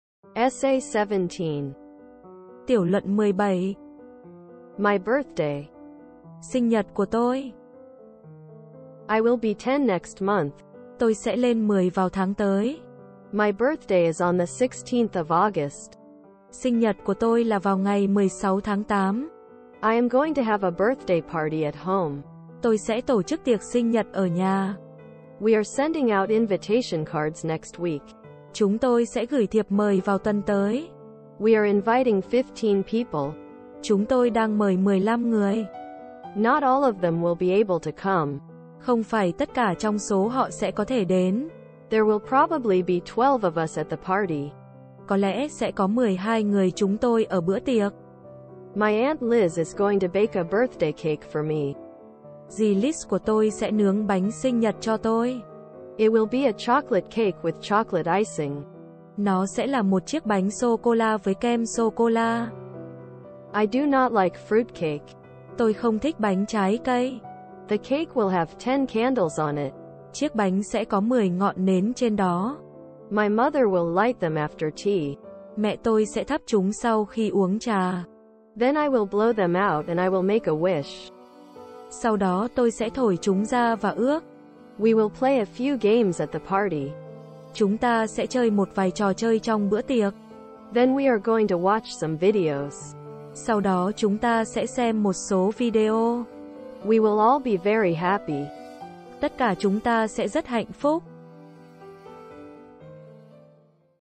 ESSAY017
My Birthday

I will be ten next month.
My birthday is on the sixteenth of August.
I am going to have a birthday party at home.
We are sending out invitation cards next week.
We are inviting fifteen people.
Not all of them will be able to come.
There will probably be twelve of us at the party.
My Aunt Liz is going to bake a birthday cake for me.
It will be a chocolate cake with chocolate icing.
I do not like fruit cake.
The cake will have ten candles on it.
My mother will light them after tea.
Then I will blow them out and I will make a wish.
We will play a few games at the party.
Then we are going to watch some videos.
We will all be very happy.

Sinh nhật của tôi

Tôi sẽ lên mười vào tháng tới.
Sinh nhật của tôi là vào ngày mười sáu tháng tám.
Tôi sẽ tổ chức tiệc sinh nhật ở nhà.
Chúng tôi sẽ gửi thiệp mời vào tuần tới.
Chúng tôi đang mời mười lăm người.
Không phải tất cả trong số họ sẽ có thể đến.
Có lẽ sẽ có mười hai người chúng tôi ở bữa tiệc.
Dì Liz của tôi sẽ nướng bánh sinh nhật cho tôi.
Nó sẽ là một chiếc bánh sô cô la với kem sô cô la.
Tôi không thích bánh trái cây.
Chiếc bánh sẽ có mười ngọn nến trên đó.
Mẹ tôi sẽ thắp chúng sau khi uống trà.
Sau đó tôi sẽ thổi chúng ra và ước.
Chúng ta sẽ chơi một vài trò chơi trong bữa tiệc.
Sau đó chúng ta sẽ xem một số video.
Tất cả chúng ta sẽ rất hạnh phúc.

adapted from:   Model Essays,  Betty Kirkpatrick and Terry Bell, 2005